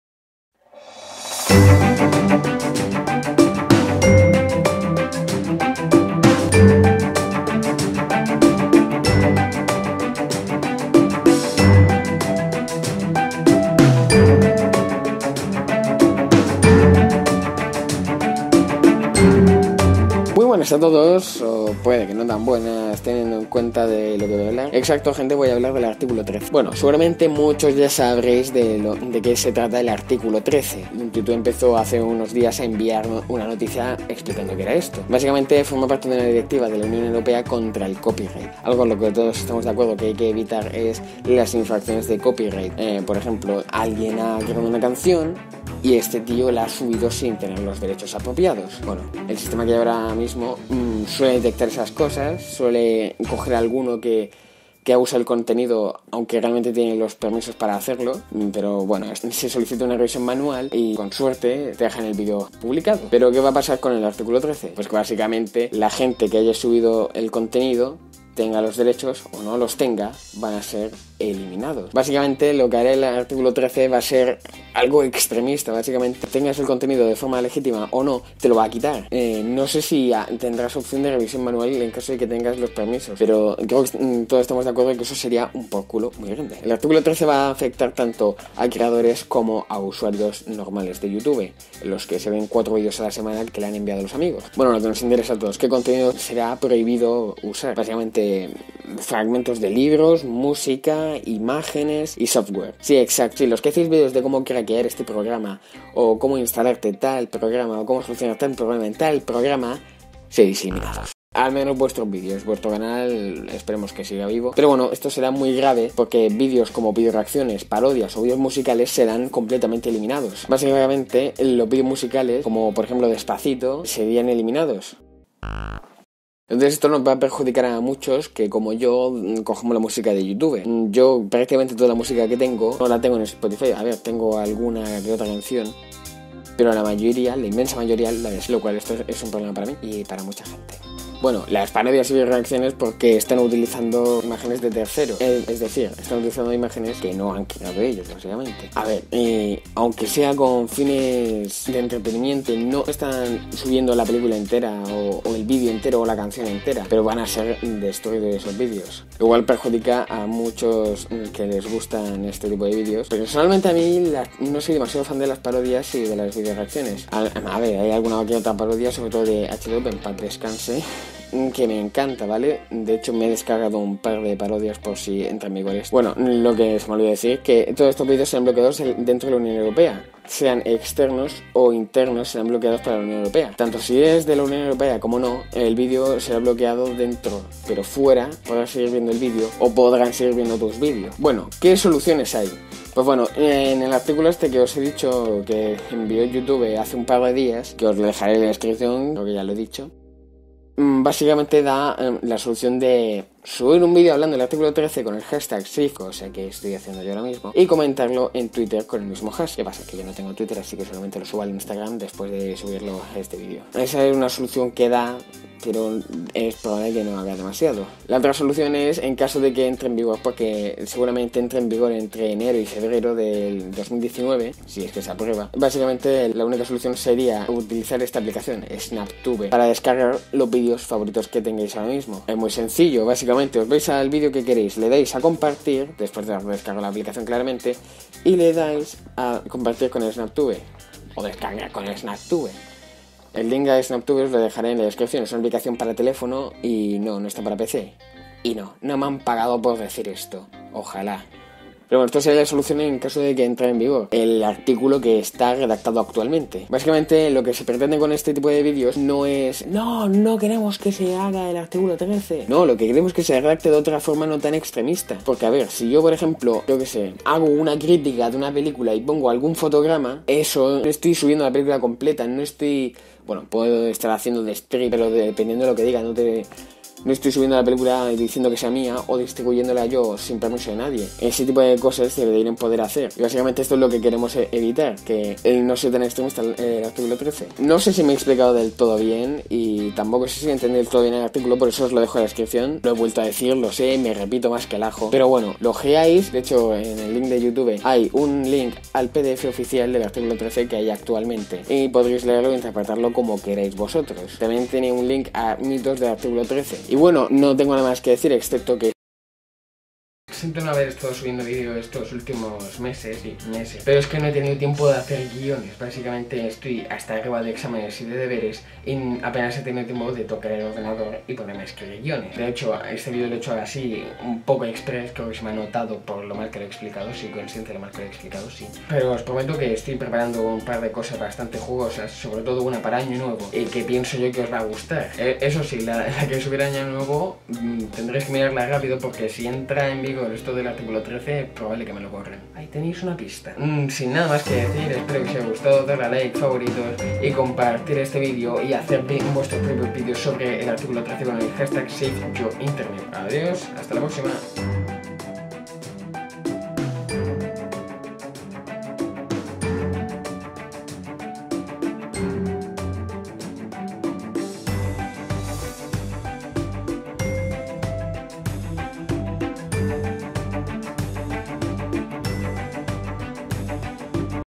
¡¡La Era oscura se acerca!! (El Articulo 13)
Gente hoy vengo a hablar de la posible era oscura que podria generar el ya famoso articulo 13

Y como me expico de p*** madre os dejho la notica de youtube donde encontrareis toda la informacion para entenderlo:

Y como solucion personal para salvar vuestros videos os dejo esta app Snaptube:

-camara (mas barata en tinda fisica)

-micro de solapa

-micro profesional (pc)